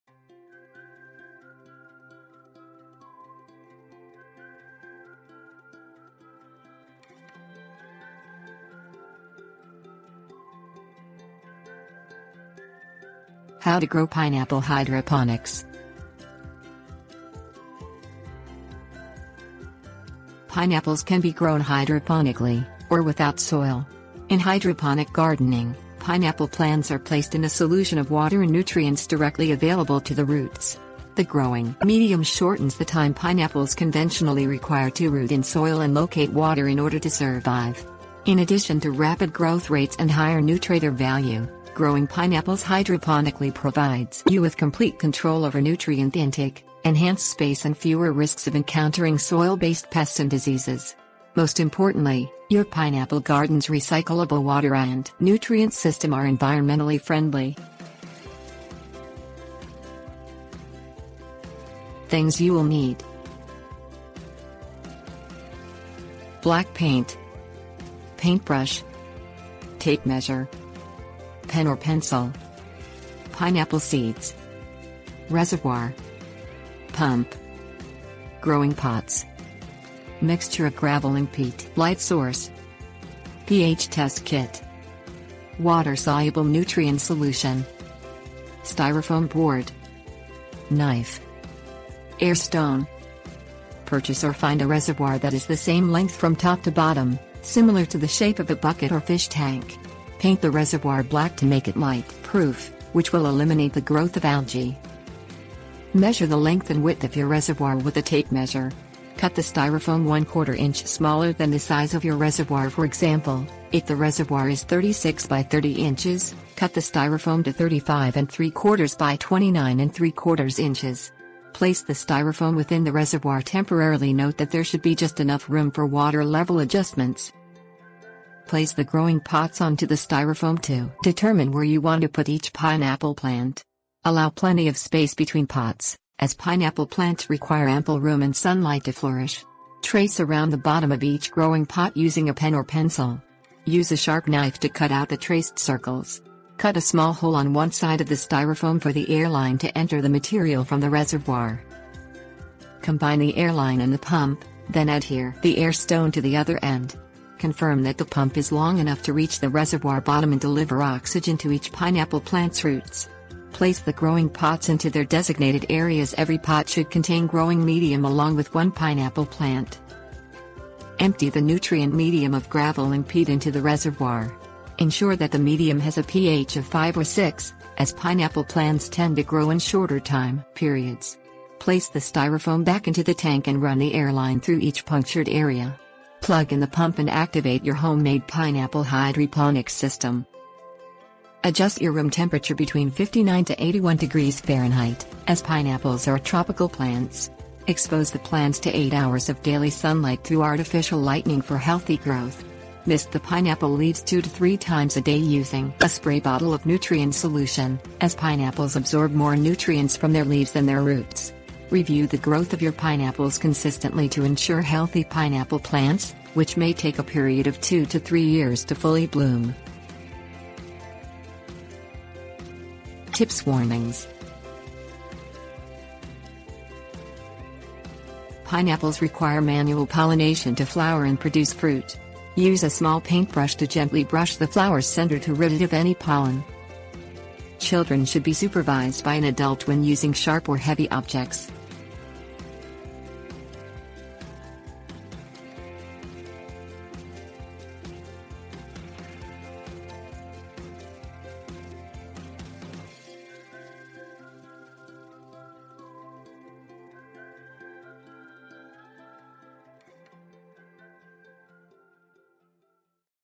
How to Grow Pineapple Hydroponics
How to Grow Pineapple Hydroponics. Pineapples can be grown hydroponically, or without soil. In hydroponic gardening, pineapple plants are placed in a solution of water and nutrients directly available to the roots. The growing medium shortens the time pineapples conventionally require to root in soil and locate water in order to survive. In...

Table of contents How to Grow Pineapple Hydroponics
Things You'll Need 01:00
Tips & Warnings 03:42